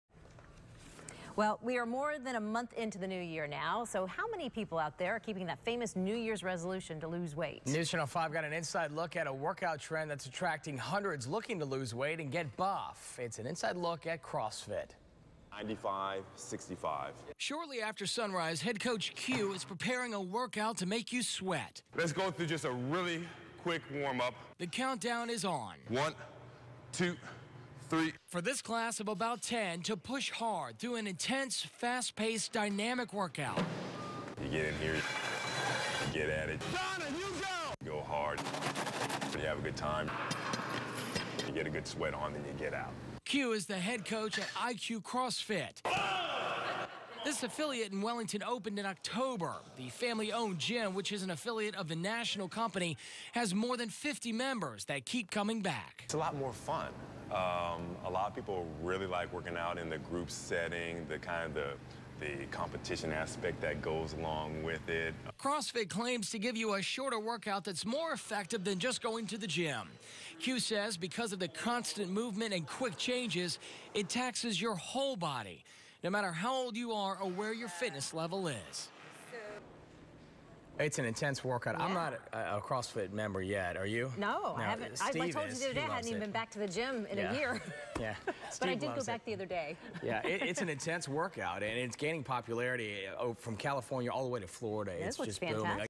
CrossFit iQ on News Channel 5
Channel 5 News in South Florida takes a look into CrossFit iQ and its style of training. CrossFit is becoming more and more popular throughout the world because of the great results you will gain. Whether you looking to loose weight, build strength or increase your endurance, CrossFit will make it happen. There are no gimmicks and no special machinery required.